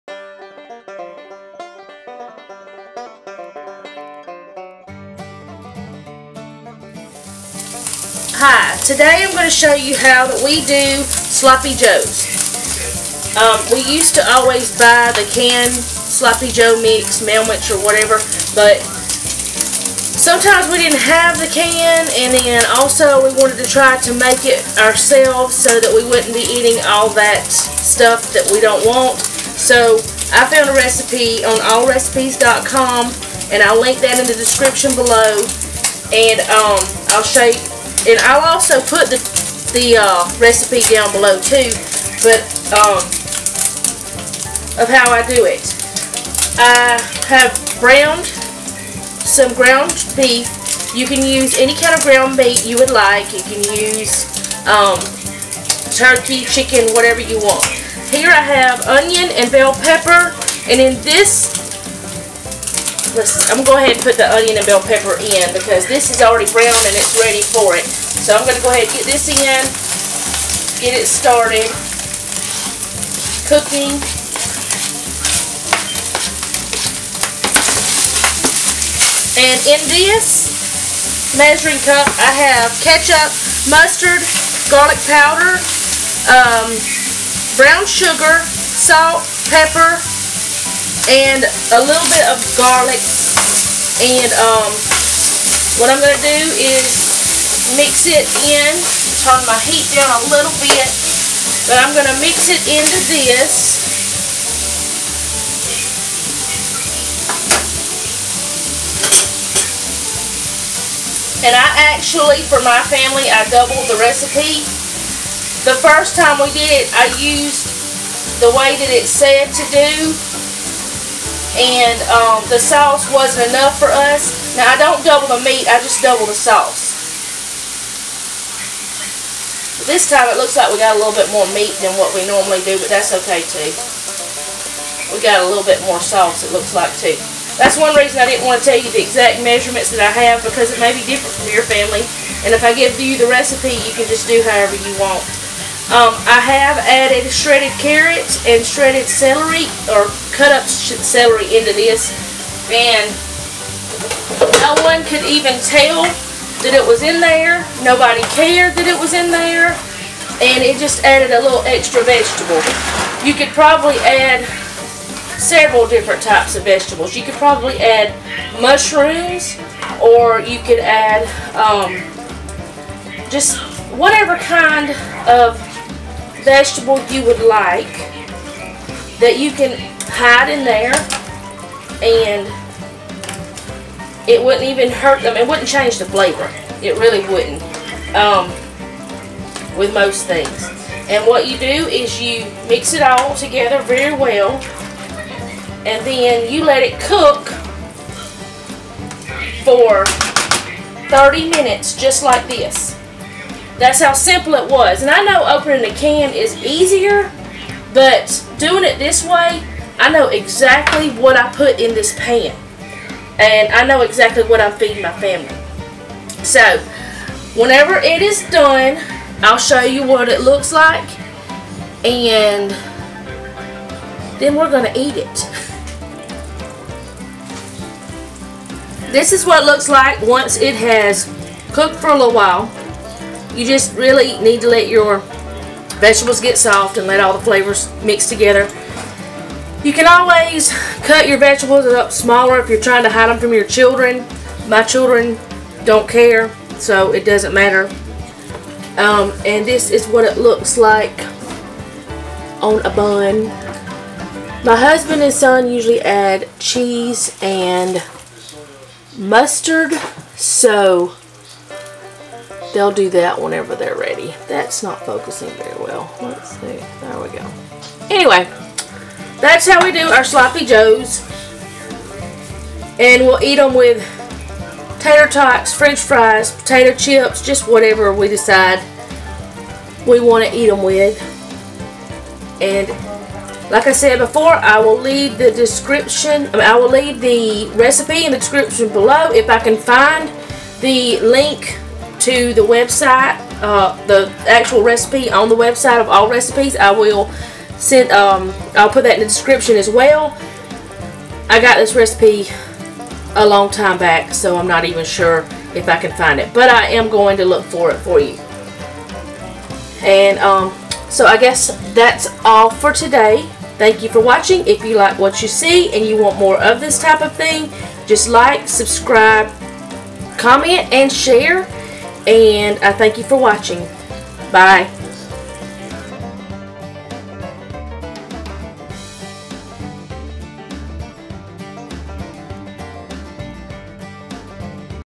Home Made Sloppy Joe
Here is my planner channel I don't PR for her, I just love her so here is Fran from @grandplans shop Amazon Storefront We decided we didn't want to eat prepackaged sauce sloppy joe anymore so I found a recipe that our family liked and I want to share it with you. It is very simple to make and here is the link to it

 Here is the recipe. I doubled the sauce because it seemed a little dry and it is better to us.

    Sloppy Joes
    1 pound lean ground beef
    1/4 cup chopped onion
    1/4 cup chopped green bell pepper
    1/2 teaspoon garlic powder
    1 teaspoon prepared yellow mustard
    3/4 cup ketchup
    3 teaspoons brown sugar